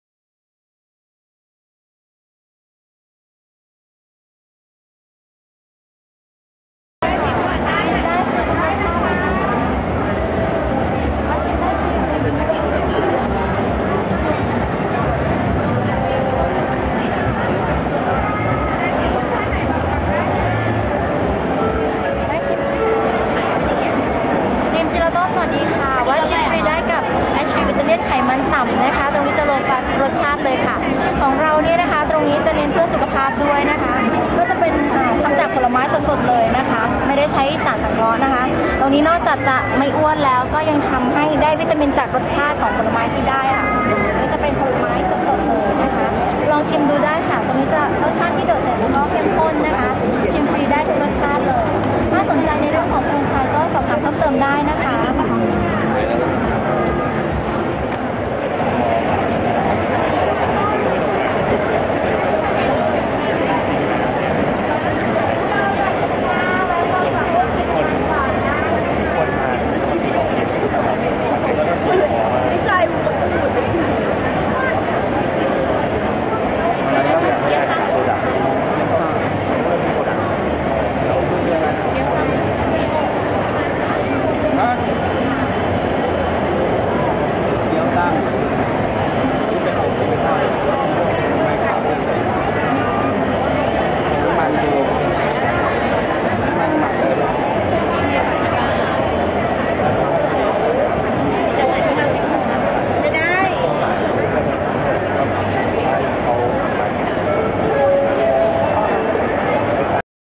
งาน TFBO 2008 ครั้งที่4
บูธไอศกรีม Dream Gelato, The Waffle, โดนัท Daddy Dough, ร้านอาหาร Tasty Thai, ร้าน Mango Tree ในตระกูลของ Coca Suki